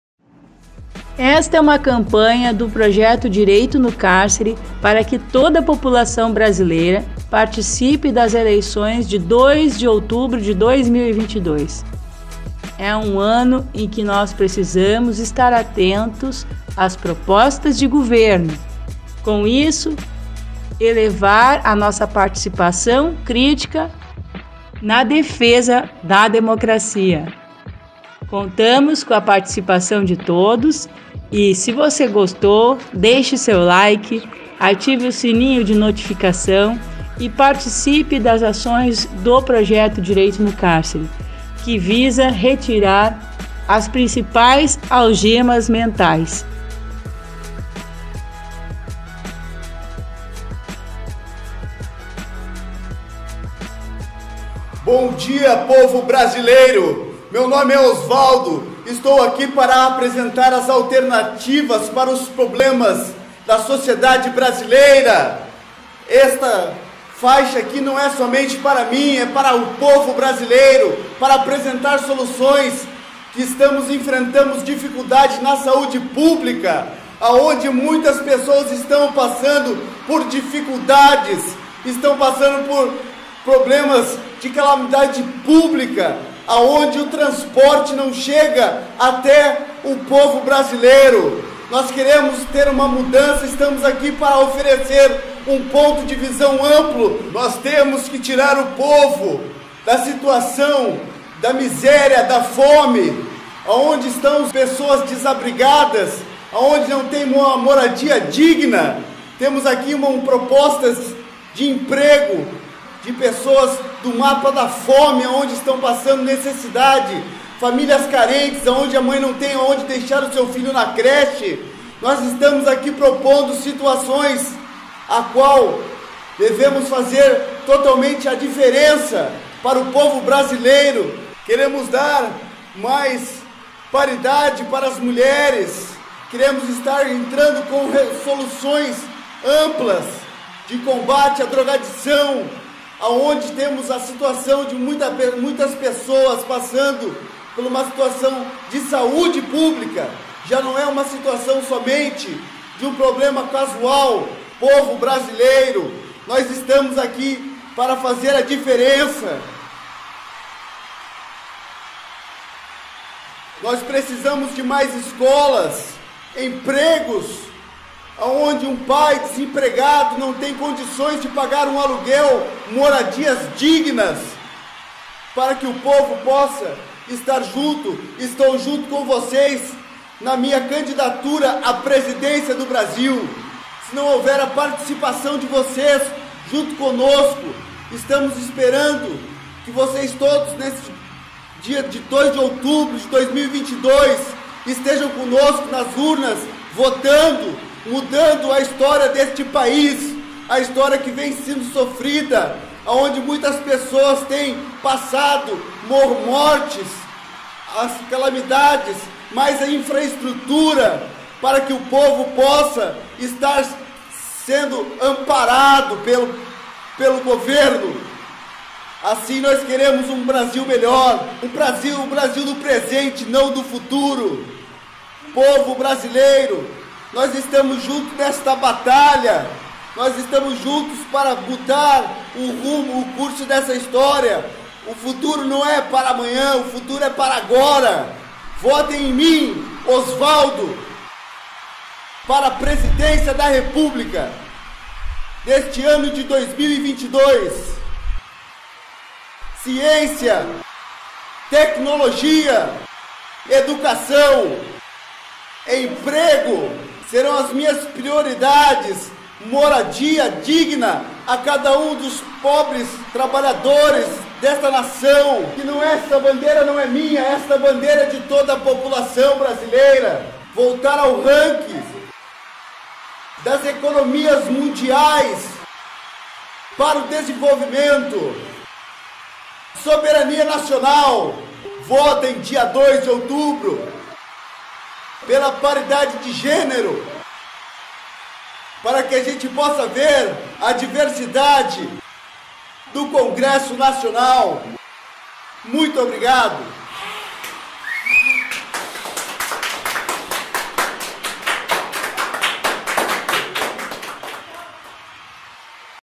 Direito no Cárcere nas Eleições 2022 - "Analise as propostas dos candidatos a cargos públicos"
Campanha Direito no Cárcere Voto Consciente. Neste ano de 2022, o Projeto Direito no Cárcere abordará através de vídeos produzidos com voluntários e detentos da Galeria E1, informações importantes sobre a participação cidadã na escolha dos candidatos a disputa: Presidente da República, governador, senador, deputados federais e deputados estaduais.

A iniciativa visa incentivar a participação pelo voto, com vistas a reduzir o número de votos nulos, brancos e abstenções, o voto consciente faz bem a democracia.

Neste primeiro vídeo alertamos sobre a importância de  ser analisado as propostas dos candidatos a cargos públicos! Então, fiquem atentos, em breve mais vídeos!